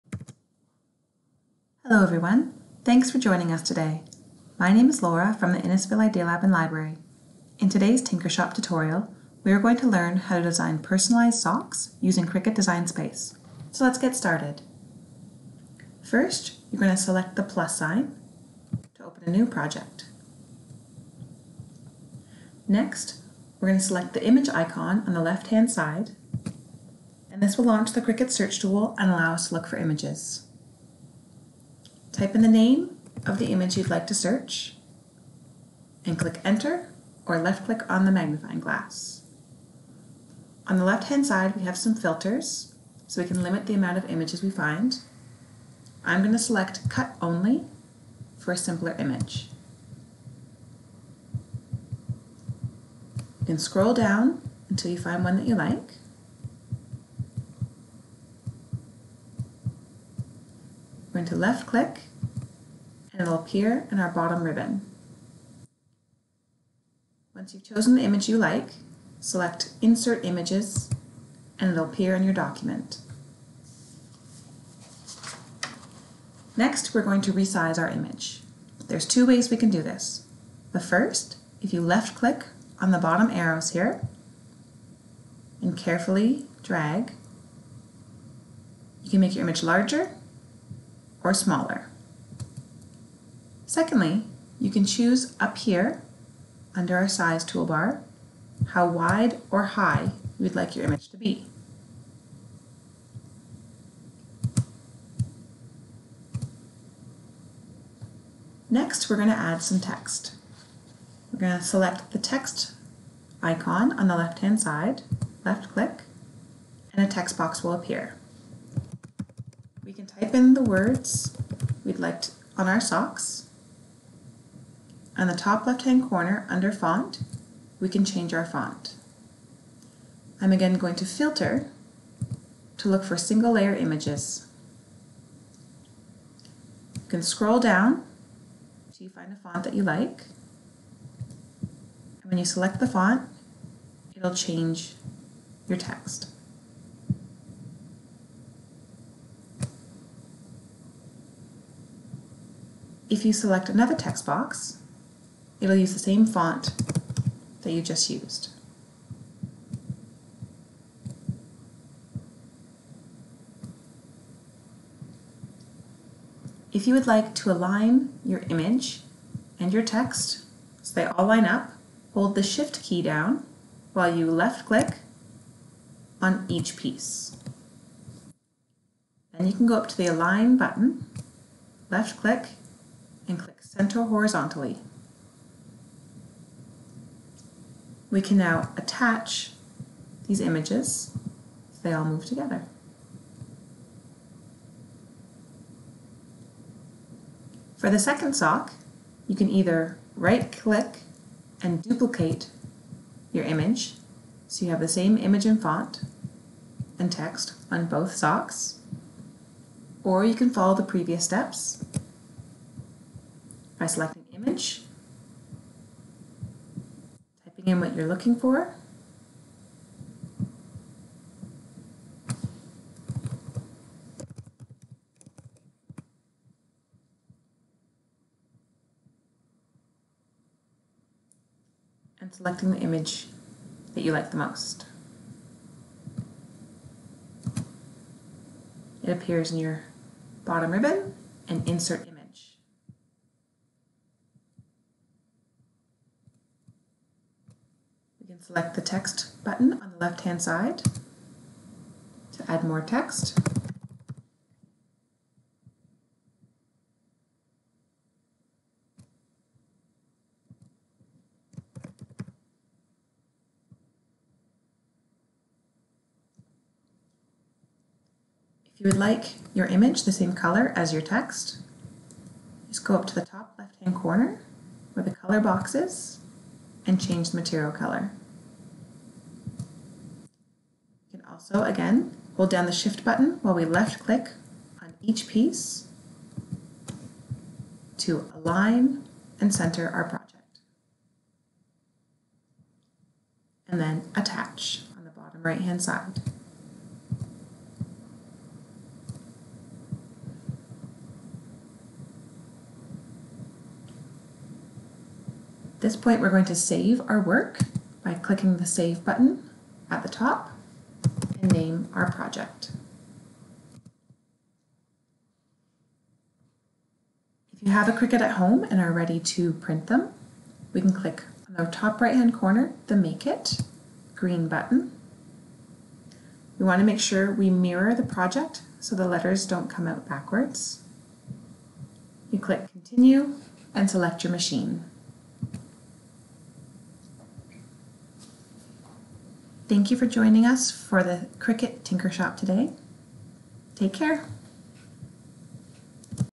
Tinkershop Tutorial: Creating Heat Transfer Decals for Cricut Socks
Do you miss designing in the hackLAB? Join our hackLAB programmer, Laura, for an online tutorial about how to design decals for socks using Cricut!